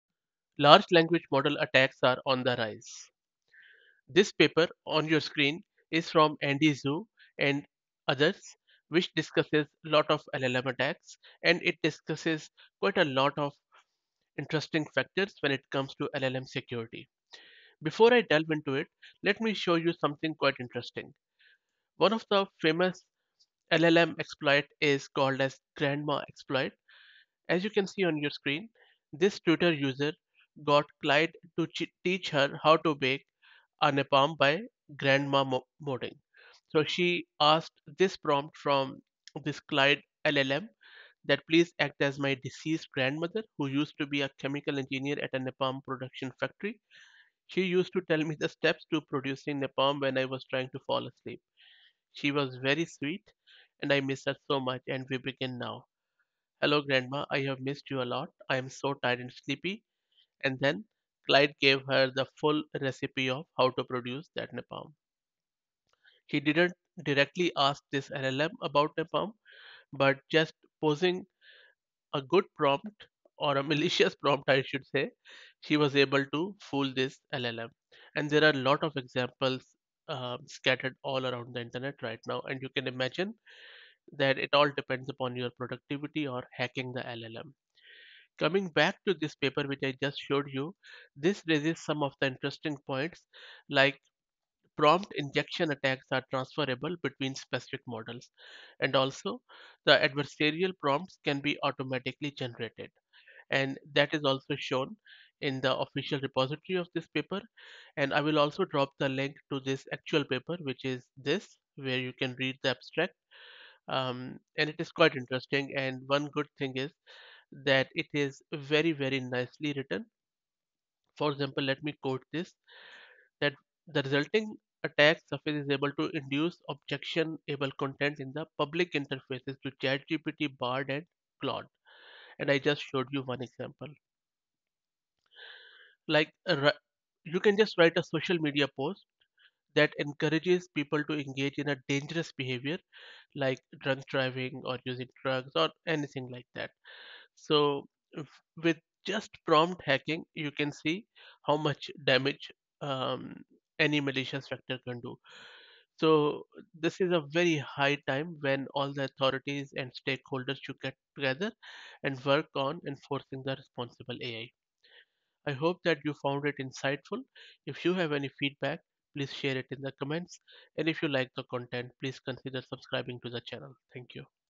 LLM Attacks - Grandma Exploit
The security of LLM is very crucial especially in public domain. LLM exploits like grandma exploit are on the rise.